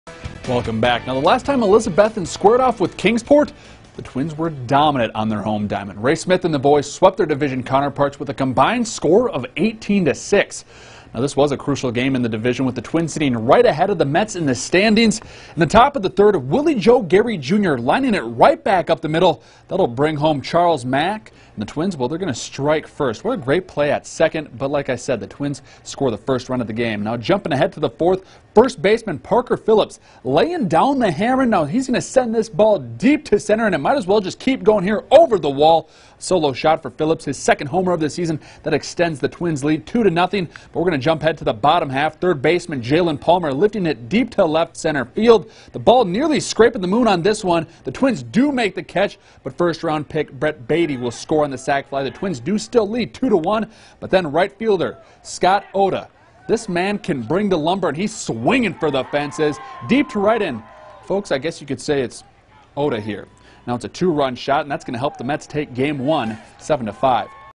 Mets Bring Down Twins
Kingsport beats Elizabethton 7-5